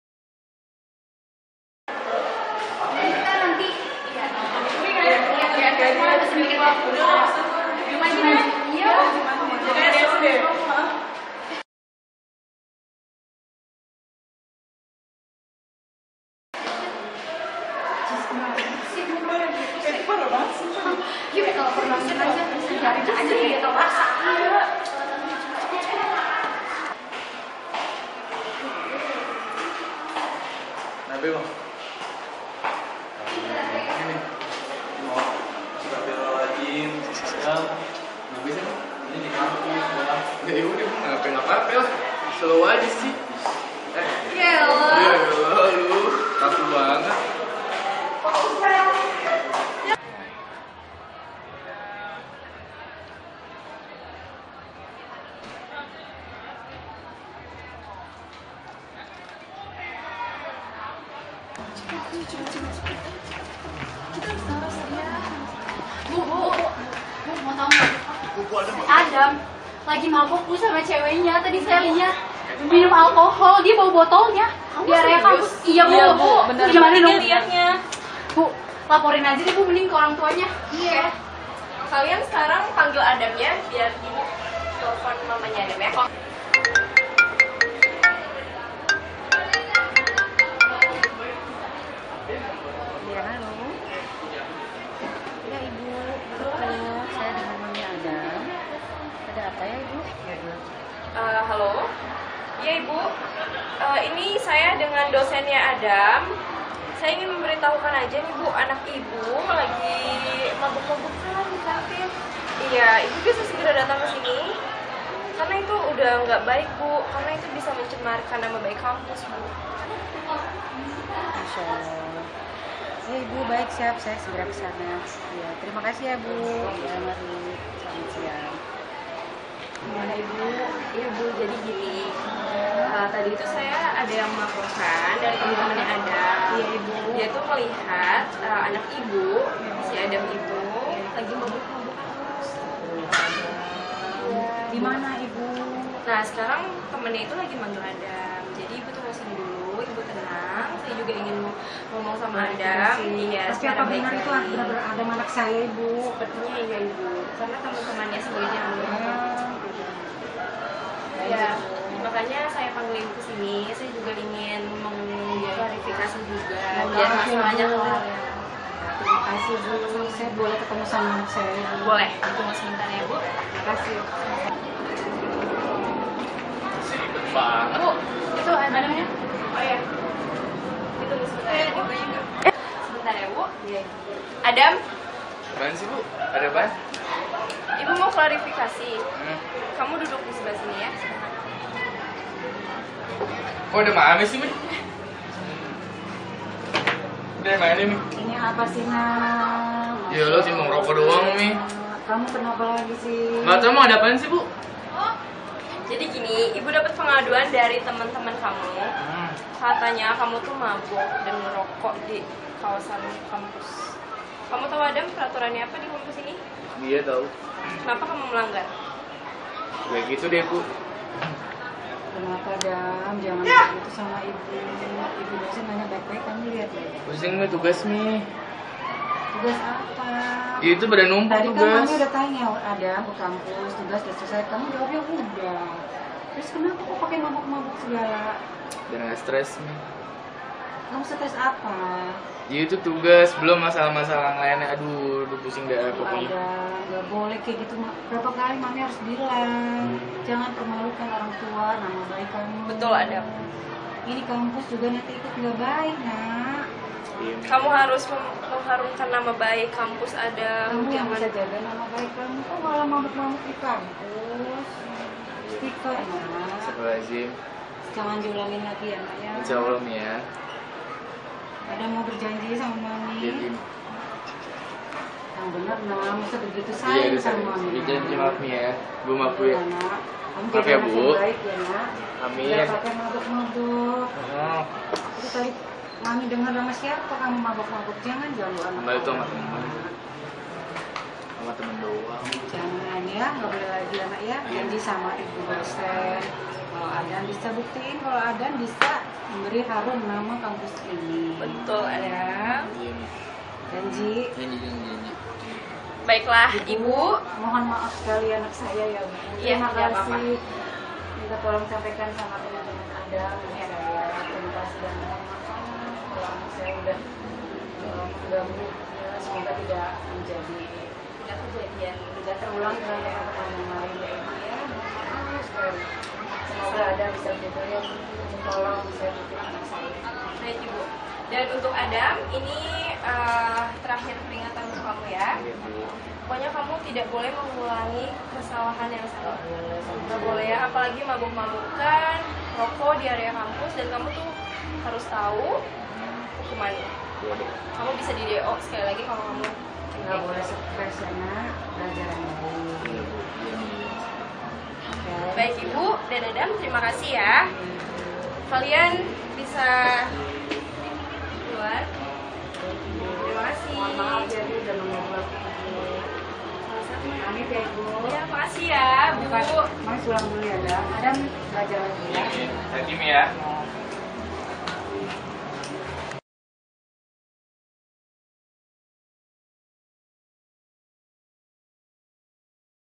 IKLAN LAYANAN MASYARAKAT | JANGAN MABUK DI KAMPUS!
Tugas Kelompok 4 Videografi Universitas Esa Unggul
Tentang iklan layanan masyarakat